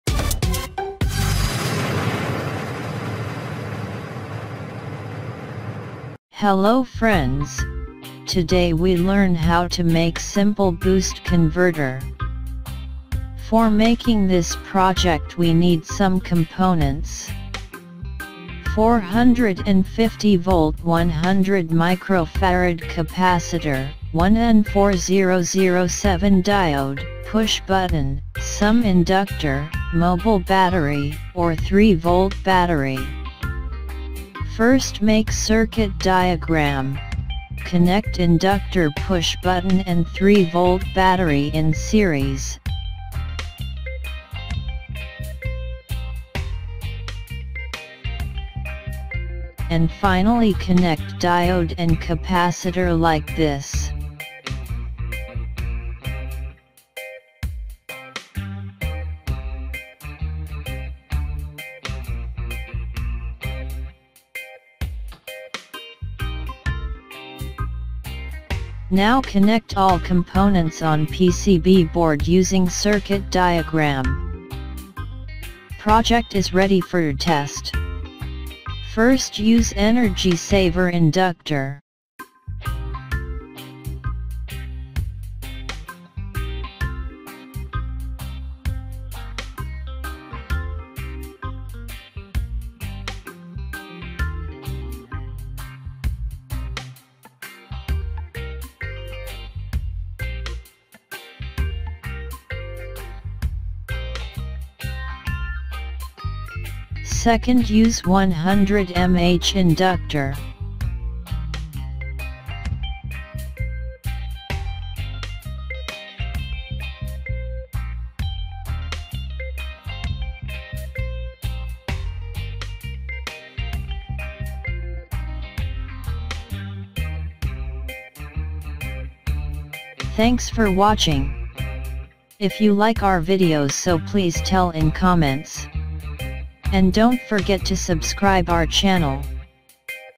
How to make simple boost converter - 3v to 1000v booster
in today video we learn how to make simple boost converter 3v to 1000v output. its very easy homemade project easy step by step with circuit diagram. its amazing voltage booster.